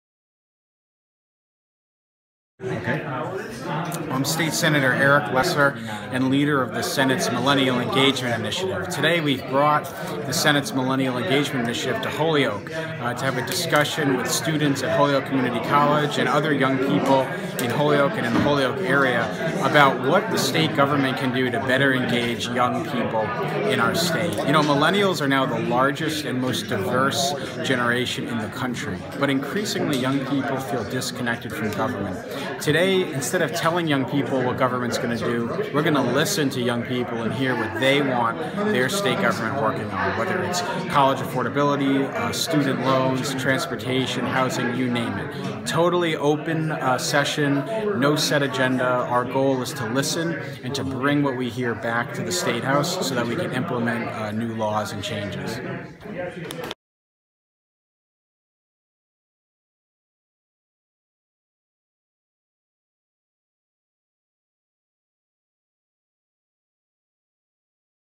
Millennial Engagement Initiative
Massachusetts state Sen. Eric Lesser, D-Longmeadow, discusses the Senate's "Millennial Engagement Initiative" in a stop at Holyoke Community College Monday, April 11, 2016.